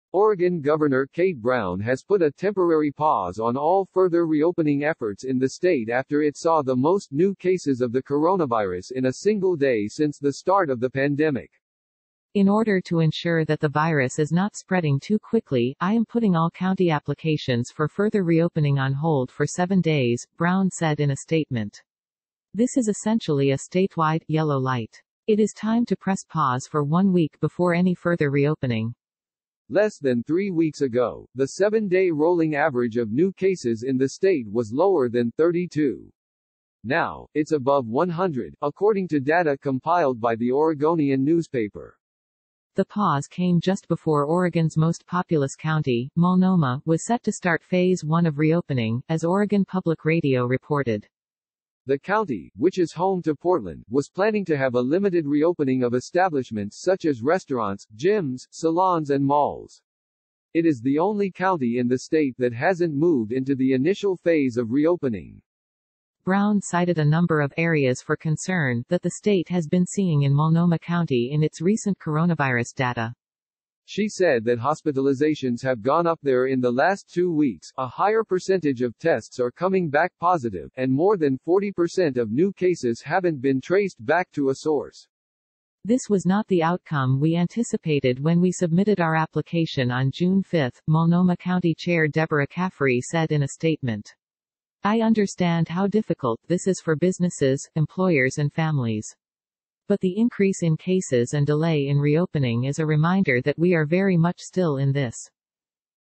Oregon Pauses Reopening As Coronavirus Cases Rise : Coronavirus Live Updates
Oregon Gov. Kate Brown has put a temporary pause on all further reopening efforts in the state after it saw the most new cases of the coronavirus in a single day since the start of the pandemic.